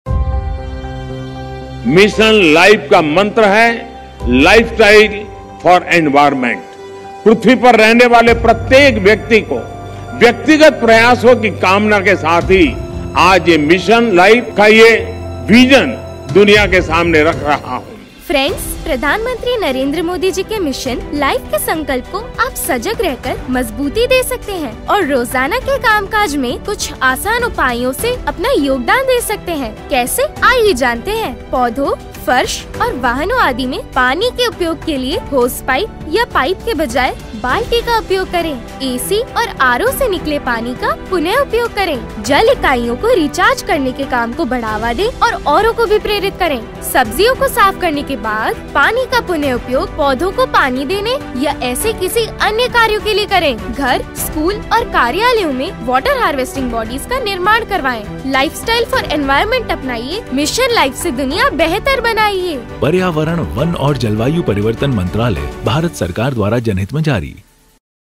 Short Film on Mission Lifestyle For Environment (LiFE) | MoEFCC | Clean & Green Panchayat | NPAW2023
Hon'ble President of India Smt. Droupadi Murmu graced the National Panchayat Awards Week 2023, held from 17th to 21st April, 2023 in New Delhi to celebrate the exemplary development activities being carried out in the Panchayats across the country. Ms. Rita Khanna, Advisor, Ministry of Environment, Forest and Climate Change presented short film on Mission Lifestyle For Environment (LiFE), fulfilling the Theme 5- Clean & Green Panchayat under Localization of Sustainable Development Goals.